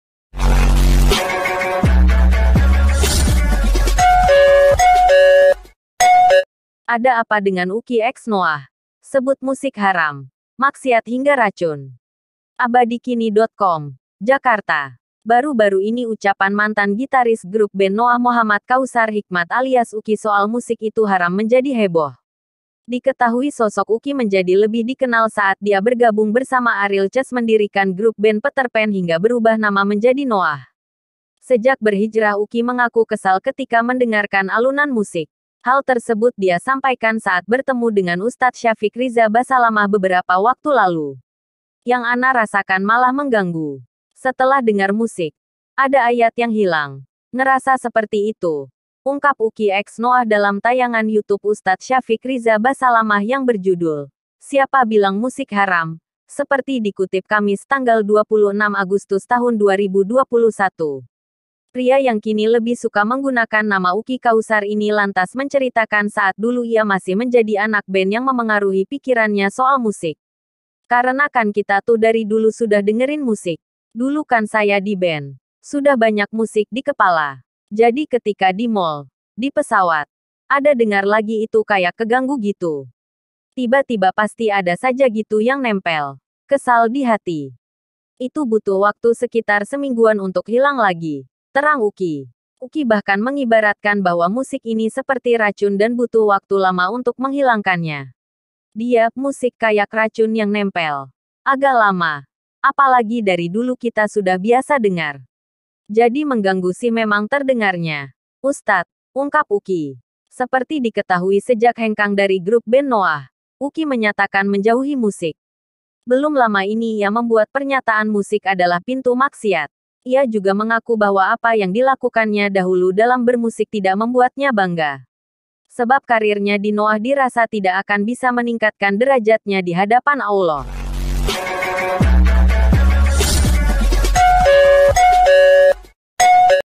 Ada Apa dengan Uki Eks NOAH? Sebut Musik Haram, Maksiat hingga Racun
Baru-baru ini ucapan mantan gitaris grup band NOAH Mohammad Kautsar Hikmat alias Uki soal musik itu haram menjadi heboh.

Diketahui sosok Uki menjadi lebih dikenal saat dia bergabung bersama Ariel cs mendirikan grup band Peterpan hingga berubah nama menjadi NOAH.

Sejak berhijrah Uki mengaku kesal ketika mendengarkan alunan musik. Hal tersebut dia sampaikan saat bertemu dengan Ustadz Syafiq Riza Basalamah beberapa waktu lalu.

“Yang ana rasakan malah mengganggu. Setelah dengar musik, ada ayat yang hilang, ngerasa seperti itu,” ungkap Uki Eks Noah dalam tayangan YouTube Ustadz Syafiq Riza Basalamah yang berjudul “Siapa Bilang Musik Haram” seperti dikutip Kamis (26/8/2021).

Pria yang kini lebih suka menggunakan nama Uki Kautsar ini lantas menceritakan saat dulu ia masih menjadi anak band yang memengaruhi pikirannya soal musik.

“Karena kan kita tuh dari dulu sudah dengerin musik. Dulu kan saya di band, sudah banyak musik di kepala. Jadi ketika di mal, di pesawat, ada dengar lagi itu kayak keganggu gitu. Tiba-tiba pasti ada saja gitu yang nempel, kesal di hati. Itu butuh waktu sekitar semingguan untuk hilang lagi,” terang Uki.

Uki bahkan mengibaratkan bahwa musik ini seperti racun dan butuh waktu lama untuk menghilangkannya.

“Dia (musik) kayak racun yang nempel, agak lama, apalagi dari dulu kita sudah biasa dengar. Jadi mengganggu sih memang terdengarnya, ustadz,” ungkap Uki.

Seperti diketahui sejak hengkang dari grup band NOAH, Uki menyatakan menjauhi musik. Belum lama ini ia membuat pernyataan musik adalah pintu maksiat.

Ia juga mengaku bahwa apa yang dilakukannya dahulu dalam bermusik tidak membuatnya bangga.

Sebab kariernya di NOAH dirasa tidak akan bisa meningkatkan derajatnya di hadapan Allah.